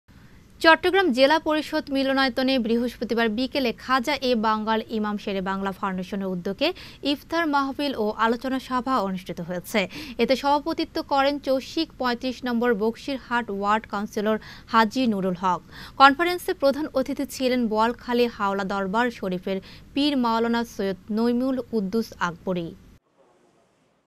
খাজা-এ বাঙ্গাল ইমাম শেরে বাংলা ফাউন্ডেশনের উদ্যোগে ইফতার মাহফিল অনুষ্ঠিত
চট্টগ্রাম জেলা পরিষদ মিলনায়তনে বৃহস্পতিবার বিকেলে খাজা-এ বাঙ্গাল ইমাম শেরে বাংলা ফাউন্ডেশনের উদ্যোগে  ইফতার মাহফিল ও আলোচনা সভা অনুষ্ঠিত হয়েছে।
এতে সভাপতিত্ব করেন চট্টগ্রাম সিটি কর্পোরেশনের ৩৫ নম্বর বক্সিরহাট ওয়ার্ড কাউন্সিলর হাজী নুরুল হক। কনফারেন্সে প্রধান অতিথি ছিলেন বোয়ালখালী হাওলা দরবার শরীফের পীর মাওলানা সৈয়দ নঈমুল কুদ্দুস আকবরী।